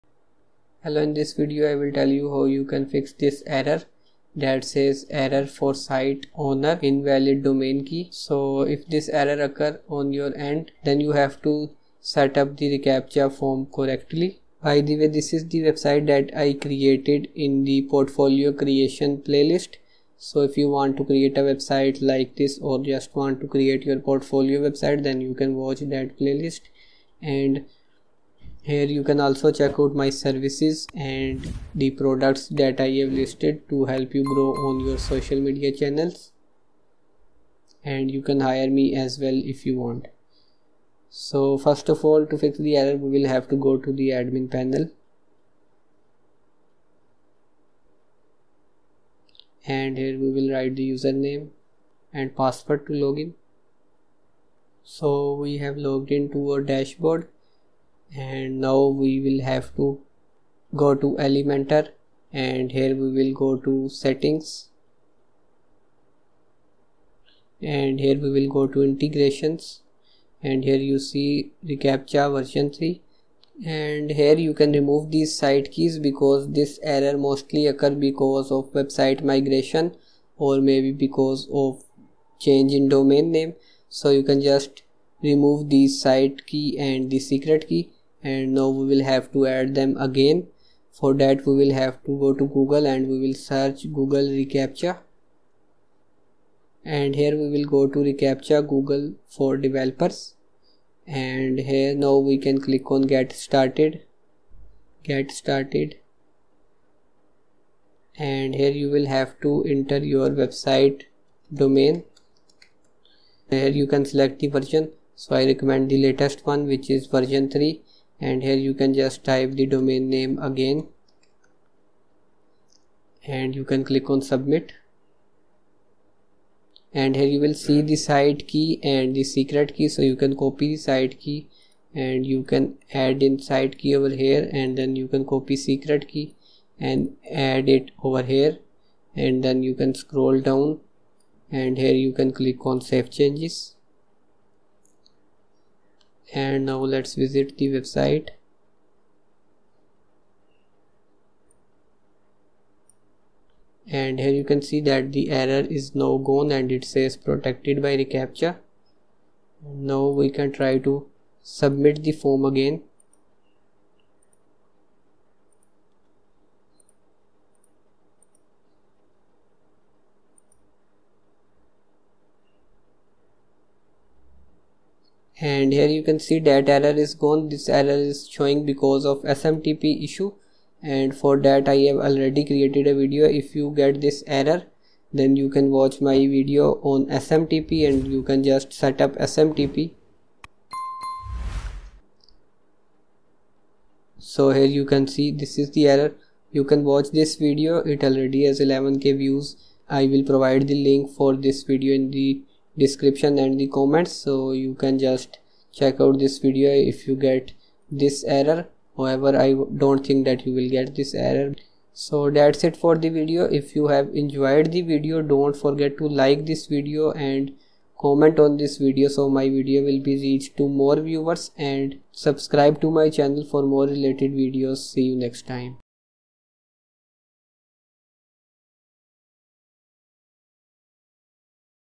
Error for Site Owner: Invalid Domain for Site Key (Fixed) - Google reCaptcha WordPress Error
Are you getting "Error for Site Owner: Invalid Domain for Site Key" issue in your WordPress website bottom right corner? This error is occurred because of invalid Google reCaptcha configuration on the website. Lets fix this issue. This video provides a step-by-step solution to correct this common issue, ensuring your site remains secure and user-friendly.

Explore my website to check my portfolio, testimonials, and services.